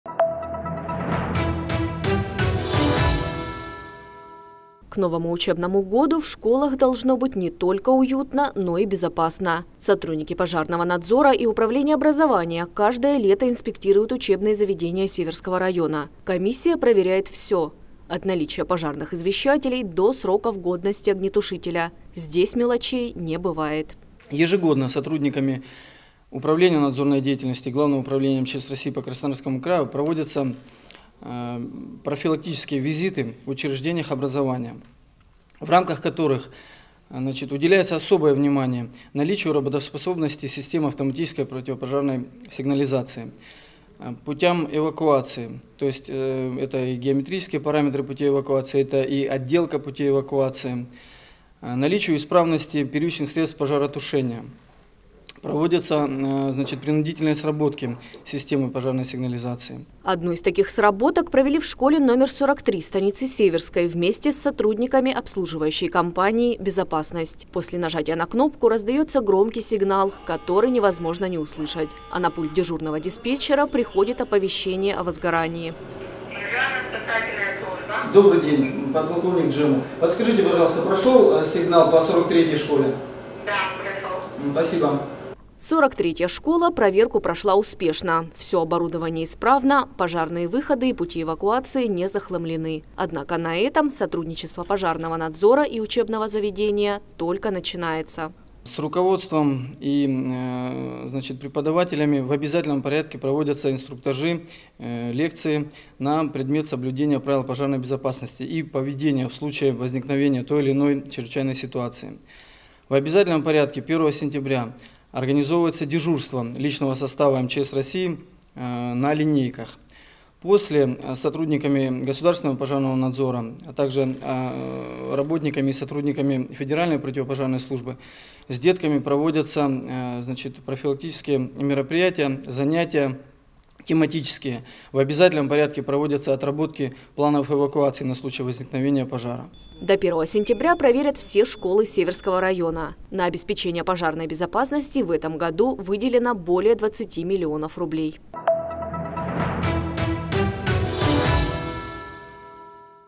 Школы Северского района проверят на пожарную безопасность
Сотрудники пожарного надзора и управления образования каждое лето инспектируют учебные заведения Северского района. Вместе с комиссией мы посетили школу № 43 в станице Северской.